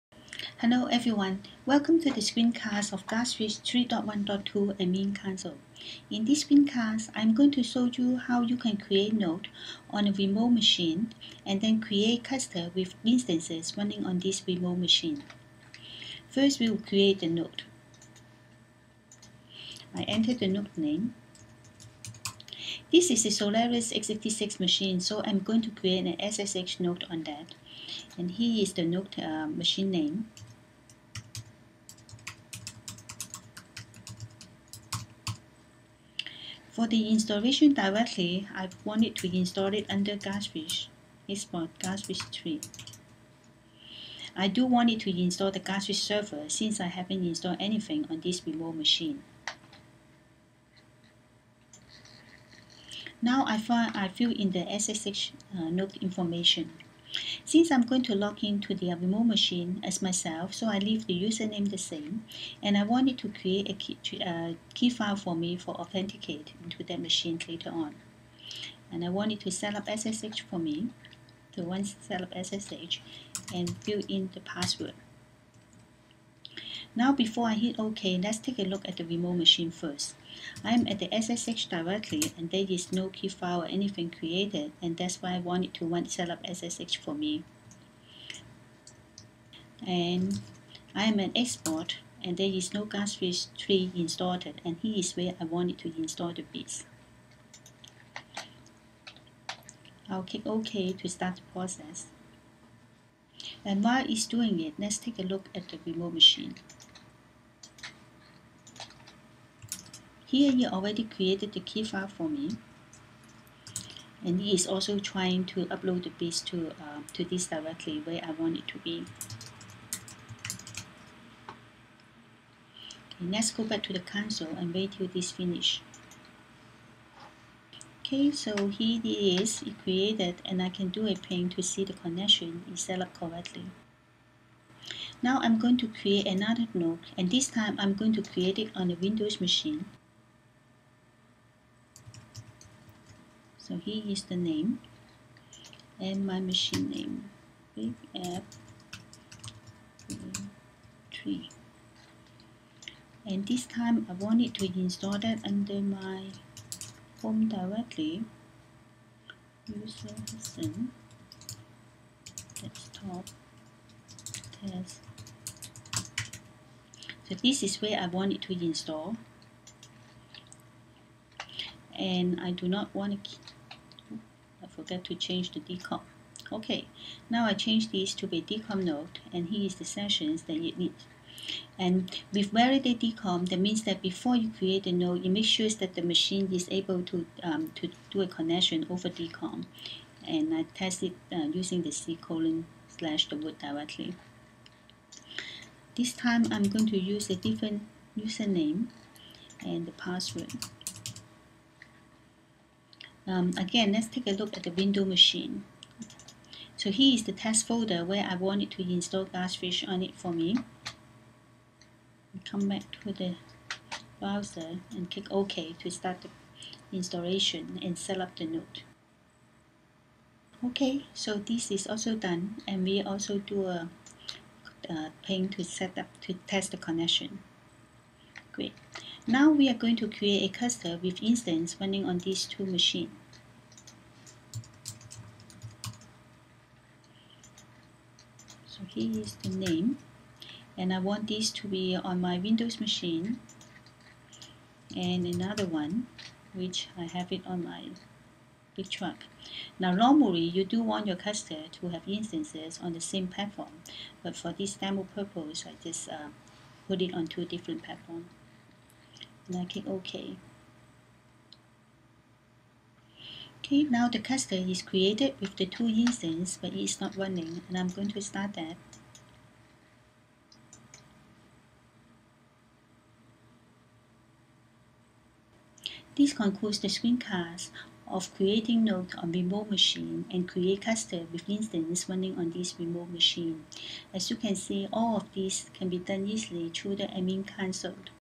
Cluster Management with GlassFish 3.1.2 Console
Cluster management with GlassFish 3.1.2 console. This shows details on how to provision GlassFish on remote machine and leverage SSH and DCOM (for windows) integration that was delivered as part of GlassFish 3.1.x release.